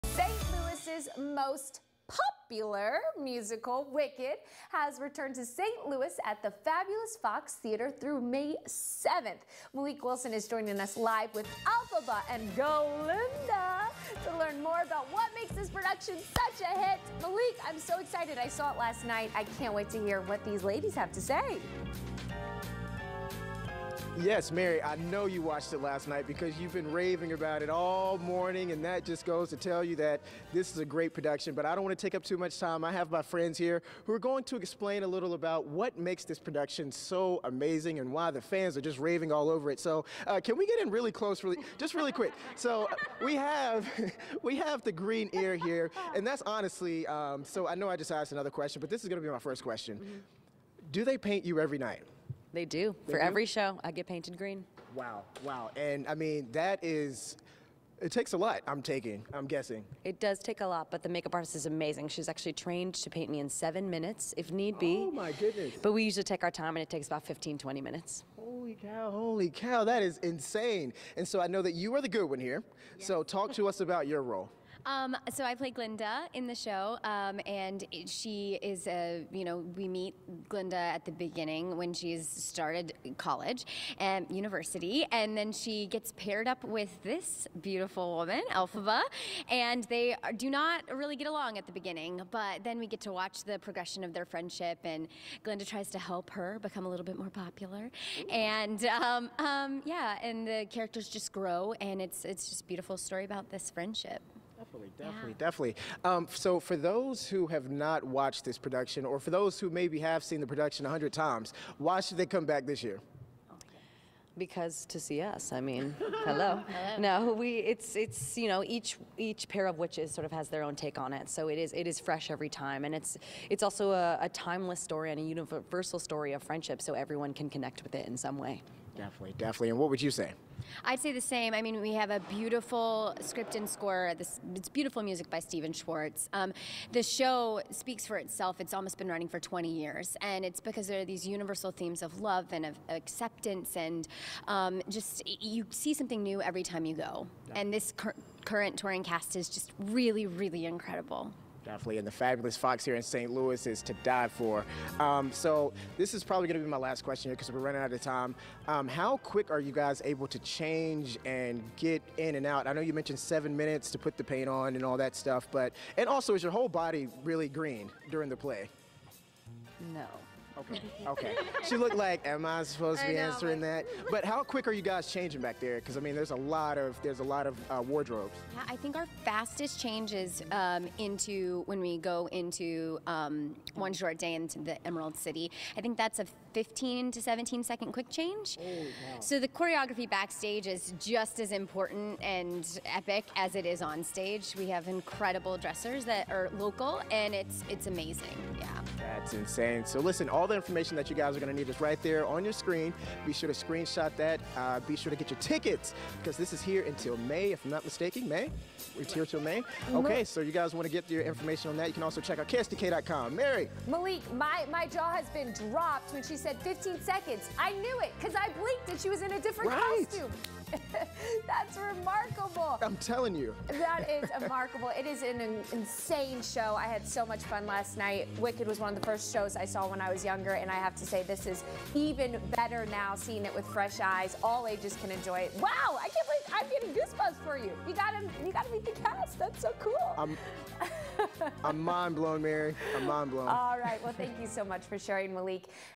Wicked to return to St. Louis at the Fabulous Fox Theatre
St. Louis’s most popular musical, will return to St. Louis at the Fabulous Fox Theatre April 12 – May 7.